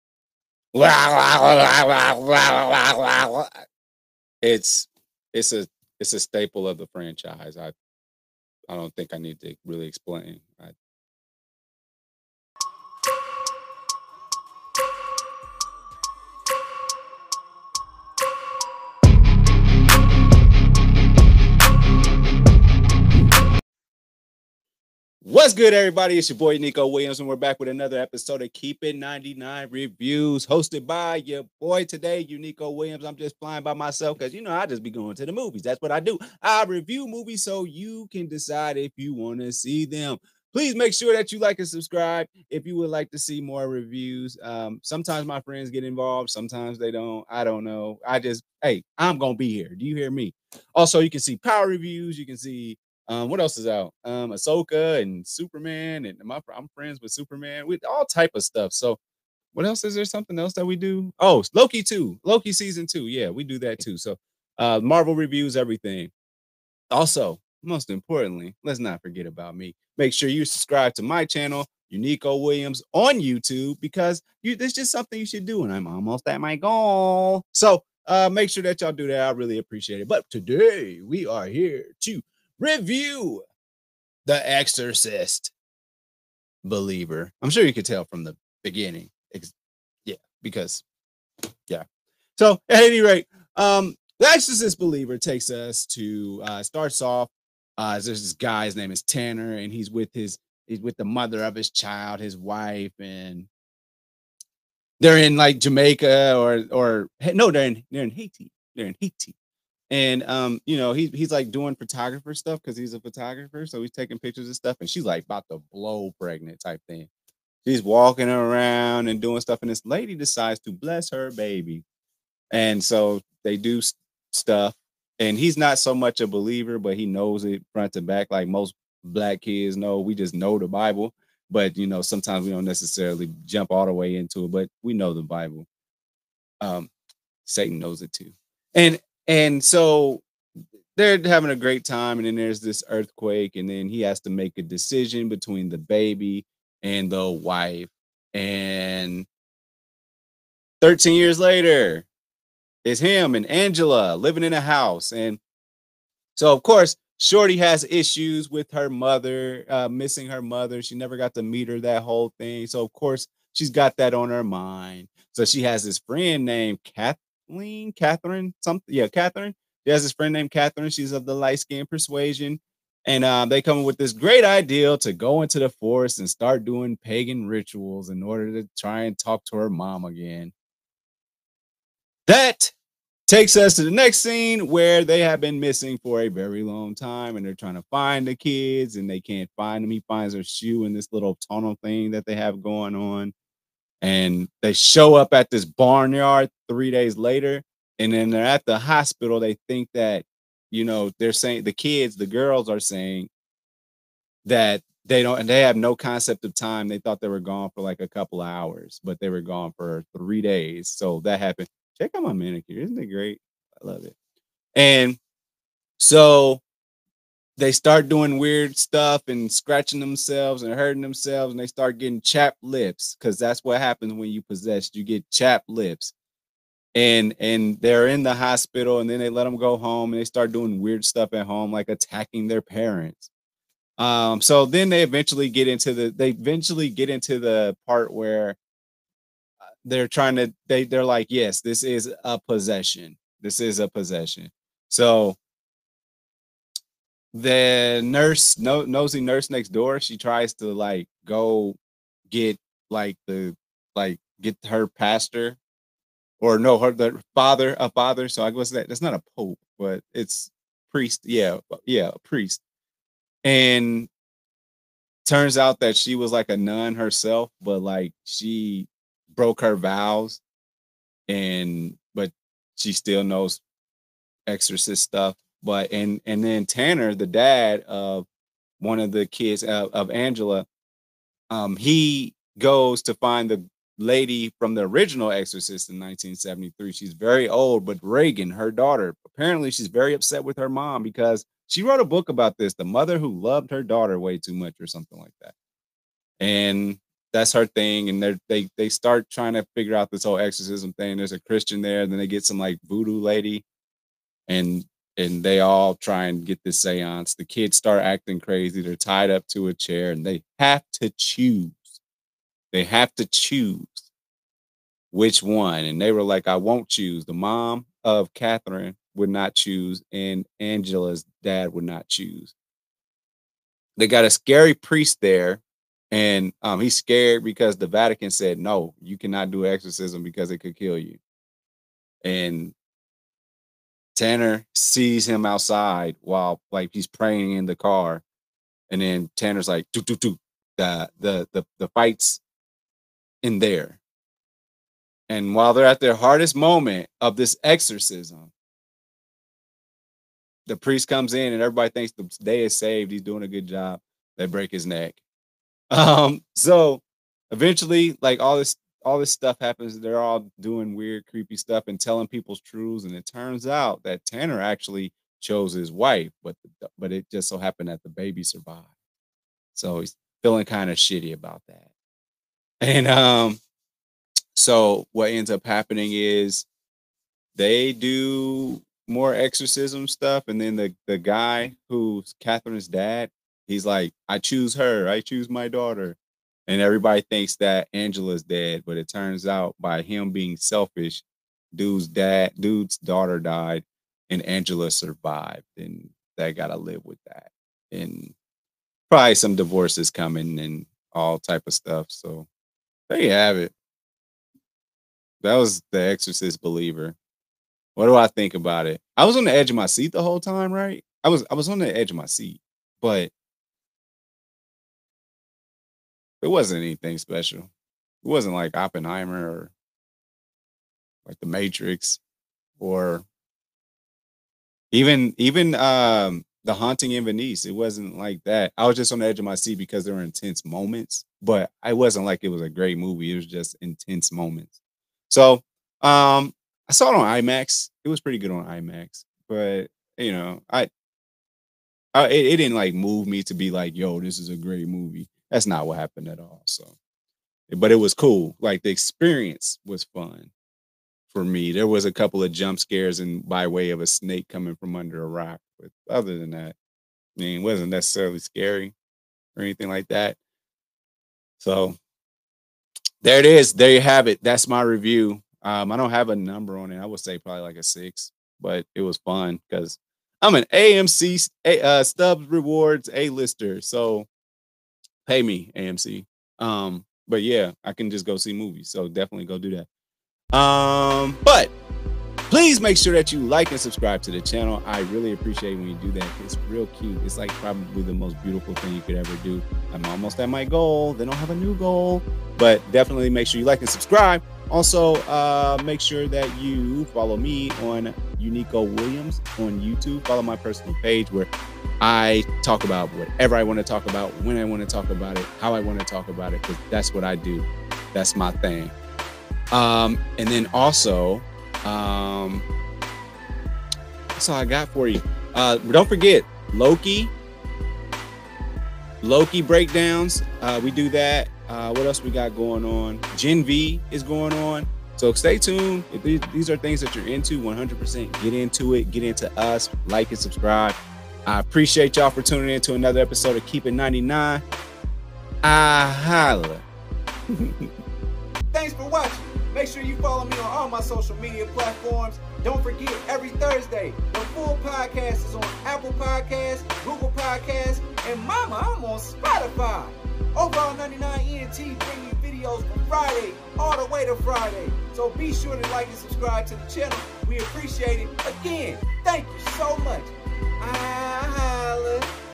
The Exorcist: Believer Movie Review | SPOILERS | Keep It 99
The Exorcist franchise is back with some fresh new faces! The beginning was intense but does the rest of the film pan out?!